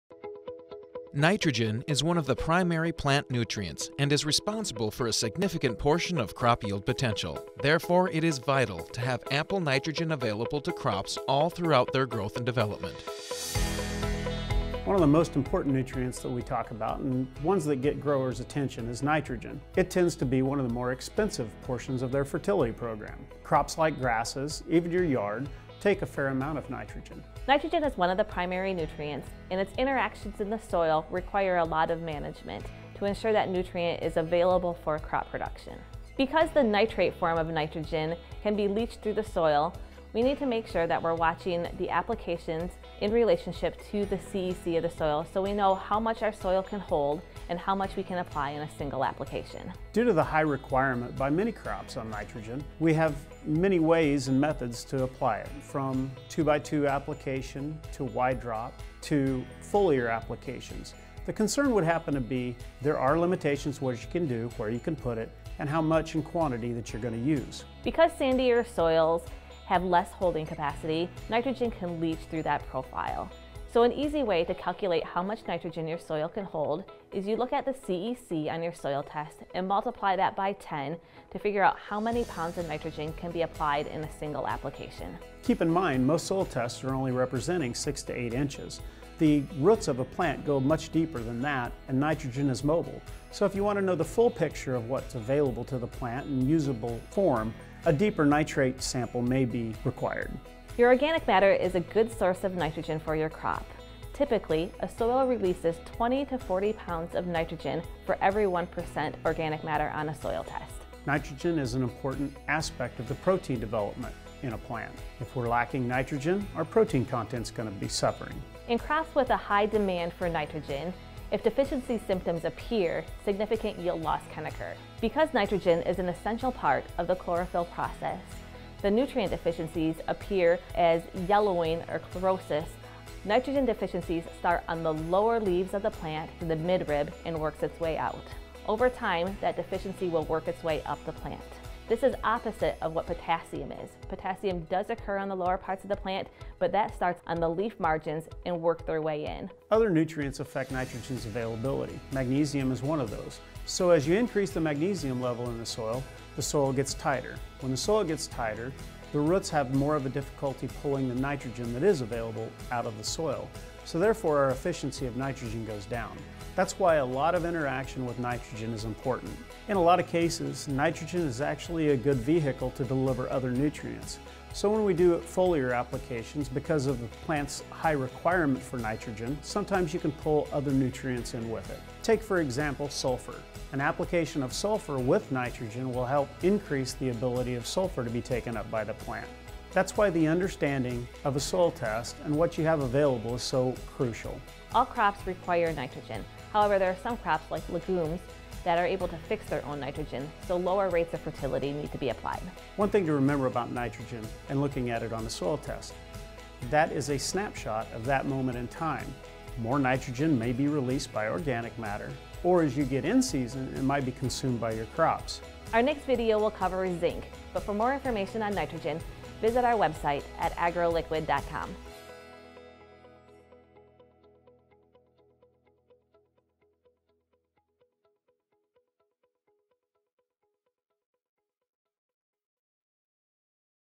Nitrogen | Back 2 Basics
Nitrogen is one of the primary plant nutrients, and probably the one we hear most about. Join Brian and Stephanie as they discuss best management practices of this critical nutrient in this Back 2 Basics video.